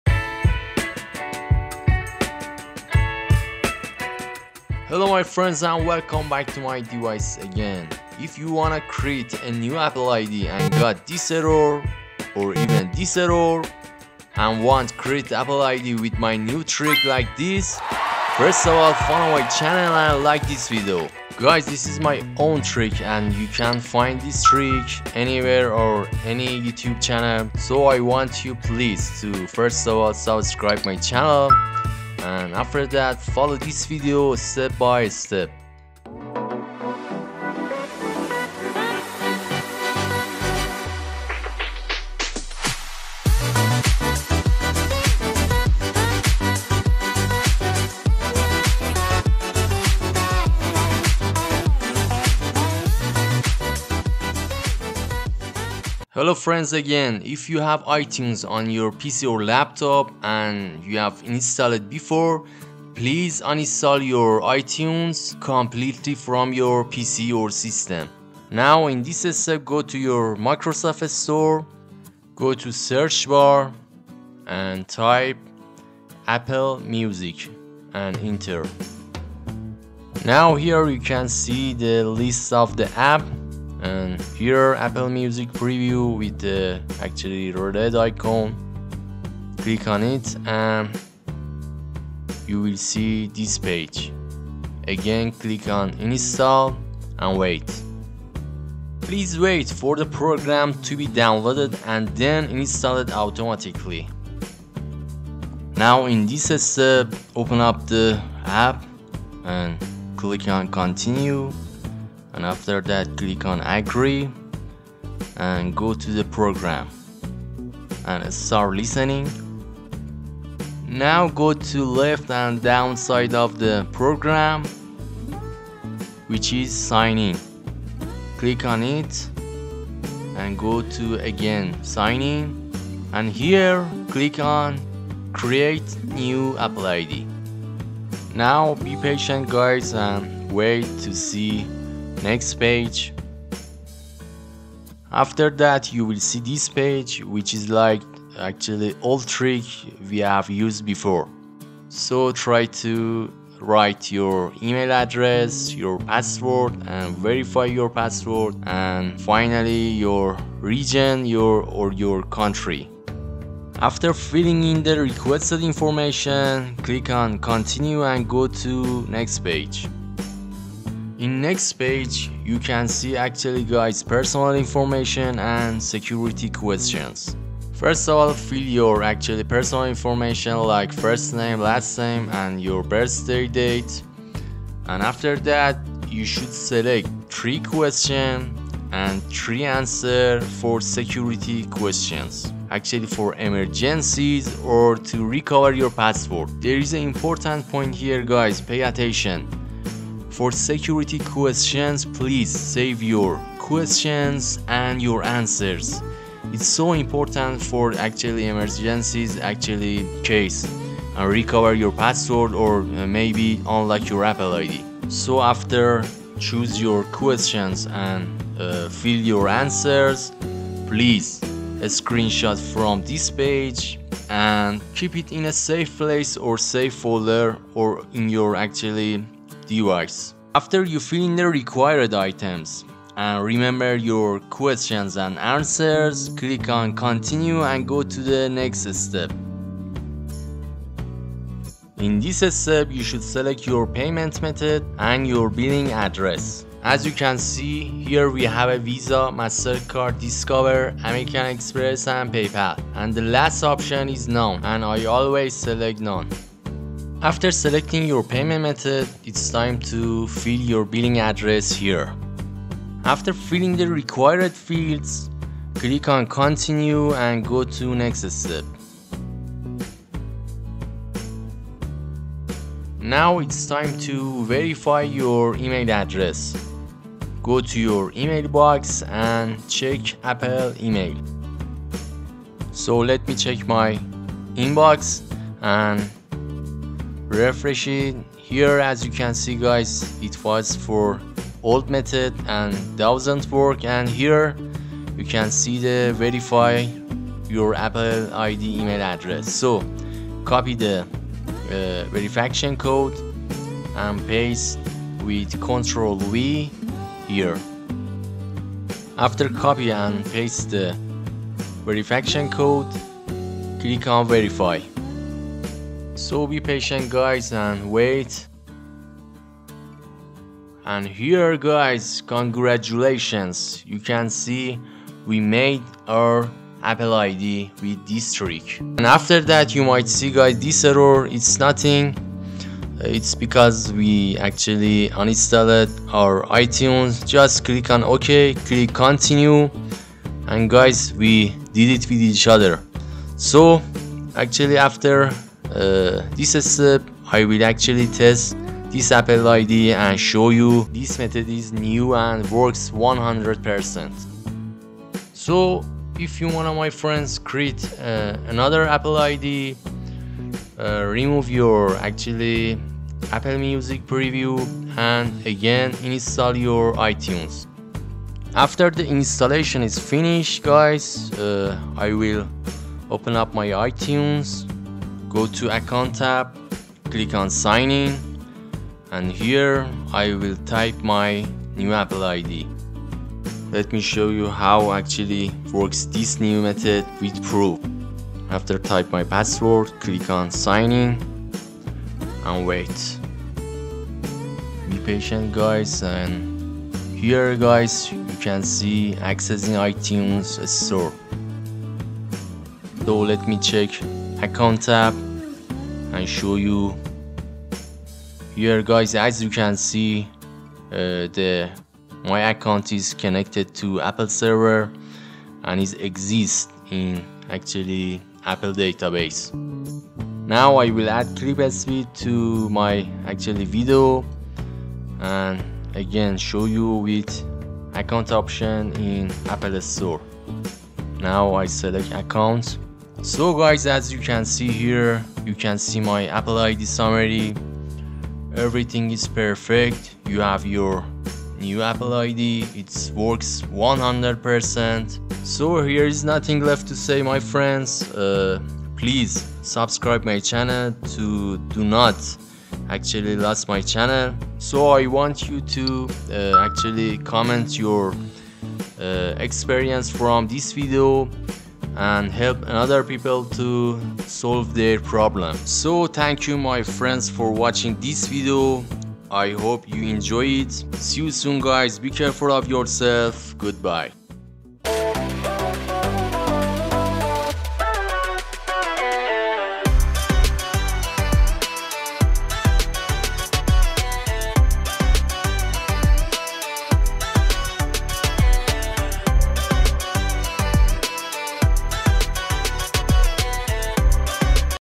How to FIX 'For assistance, contact iTunes Support' Error on iTunes
Do you get the "For assistance, contact iTunes Support error when opening iTunes? This frustrating error can prevent you from creating Apple Id.
This error can occur for a few reasons, but I will show you how to troubleshoot it and create your Apple ID without any problems.